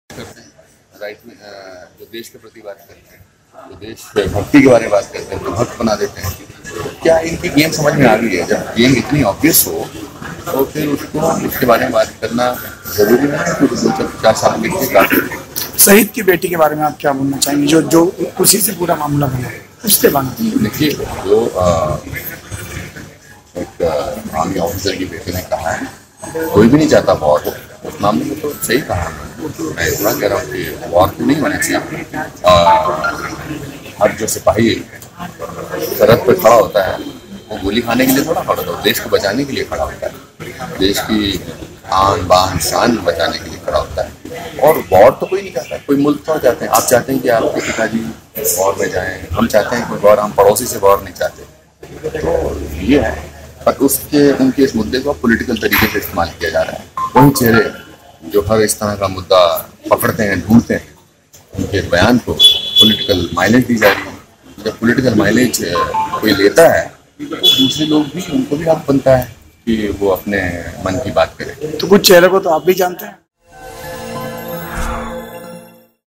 Bollywood Actor Anupam kher support gurmehar kaur from troll army
Bollywood Actor  Anupam kher support gurmehar kaur from troll army indore madhya pradesh